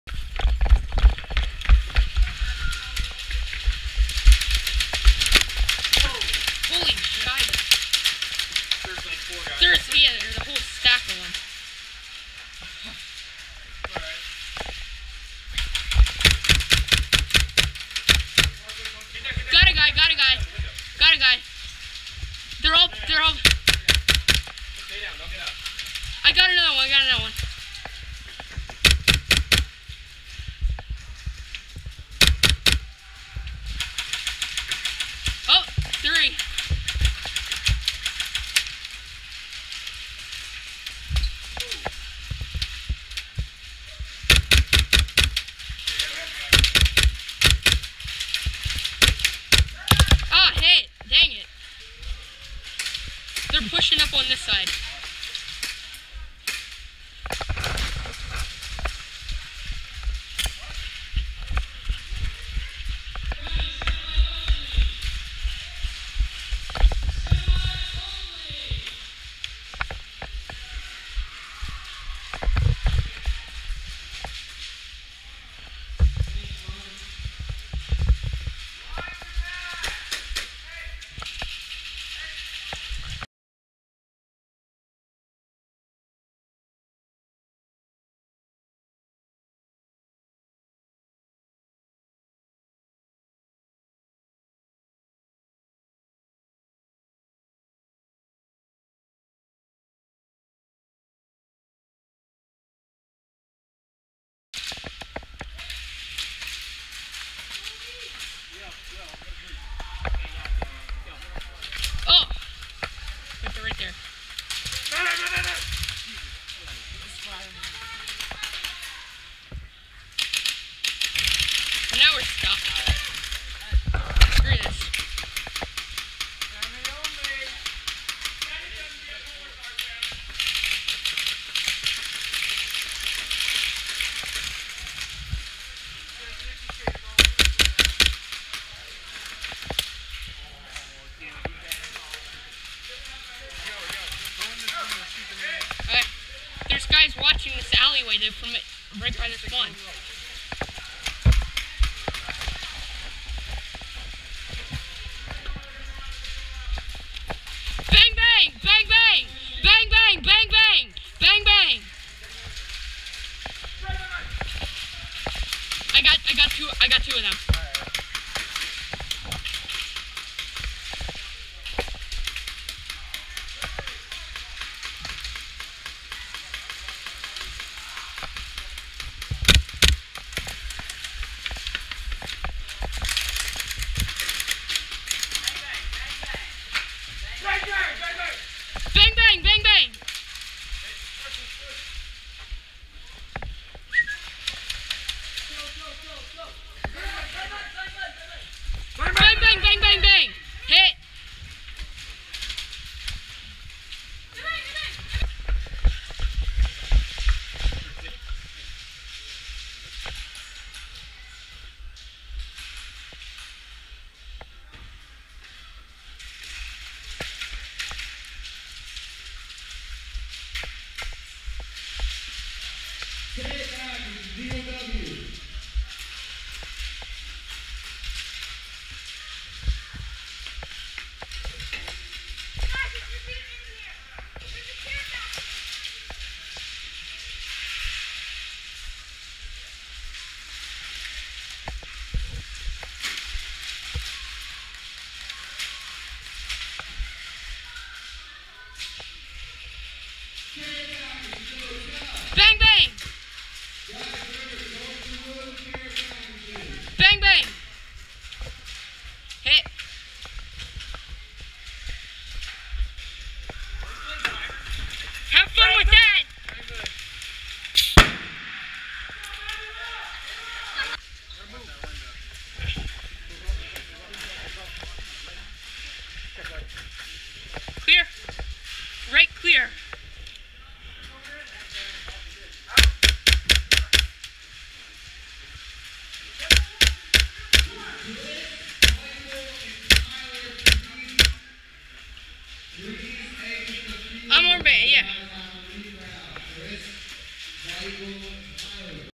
Stryker Airsoft NJ 12/26/15
Hey guys this video is some airsoft gameplay at Stryker Airsoft in NJ.